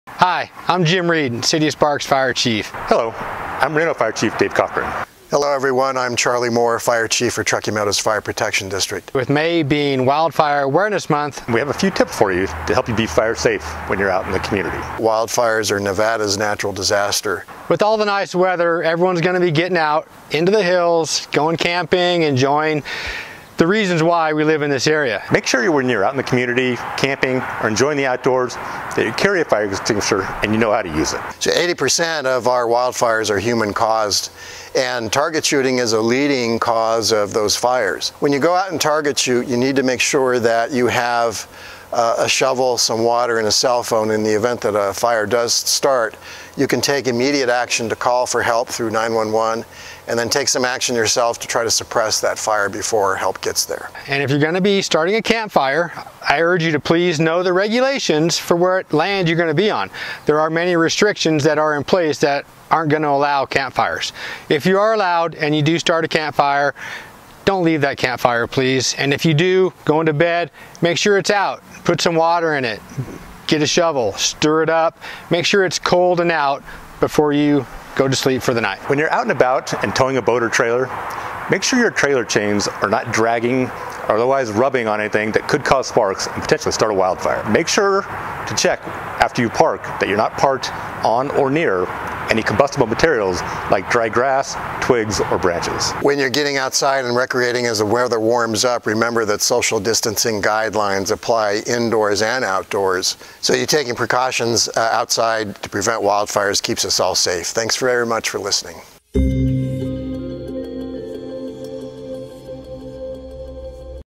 Wildfire Awareness Month May 2020
May is Wildfire Awareness Month. Fire Chiefs from Reno, Sparks and Washoe County have some great tips you can do to help prevent wildland fires.